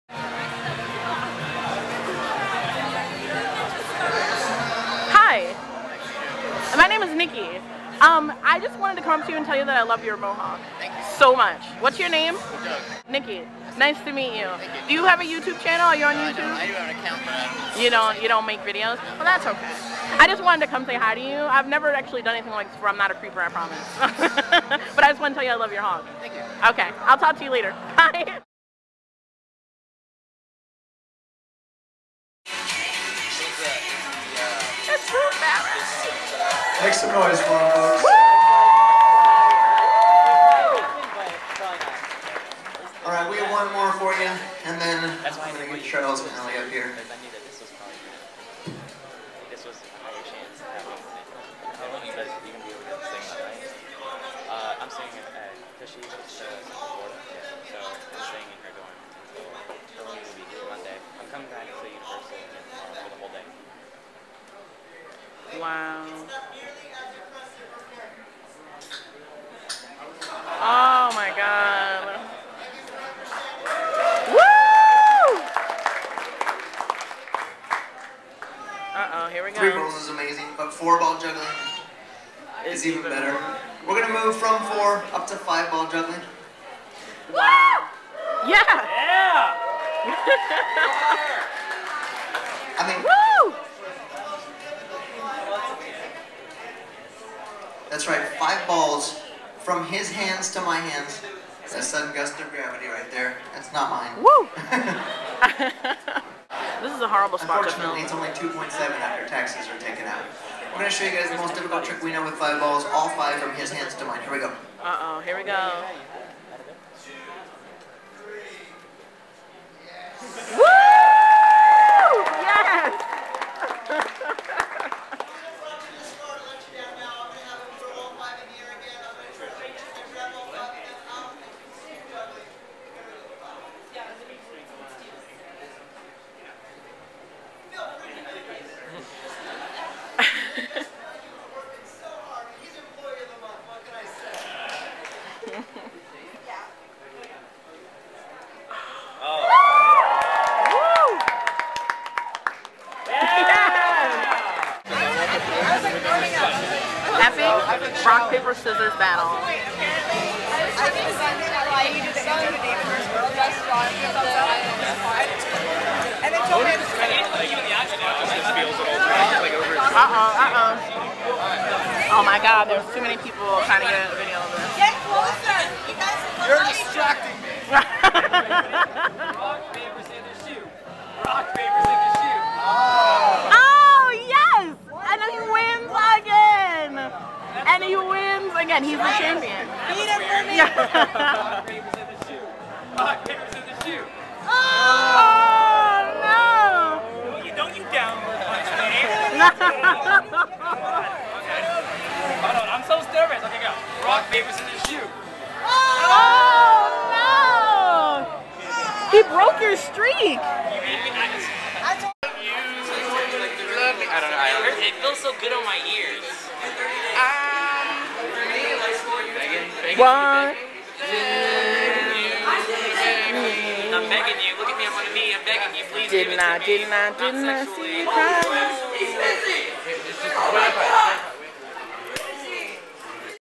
Orlando Gathering - 2010
i have very little footage from the orlando gathering in july cuz i was too busy having fun and meeting ppl. selfish i know :P

but here's the footage i do have. enjoy! :)